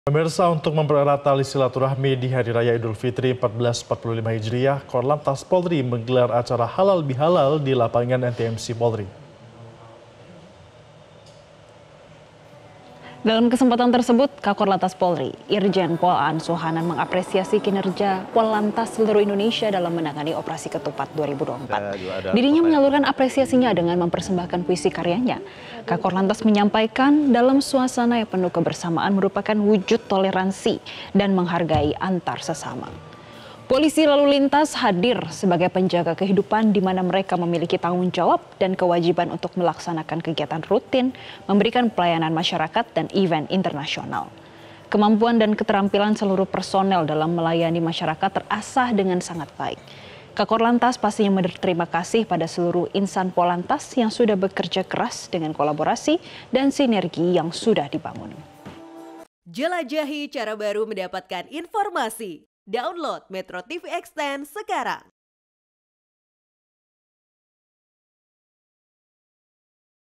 Kakorlantas Apresiasi Polantas Seluruh Indonesia
MetroTV, Untuk mempererat tali silaturahmi di hari raya Idulfitri 1445 hijriah, Korlantas Polri menggelar acara halal bihalal di lapangan NTMC Polri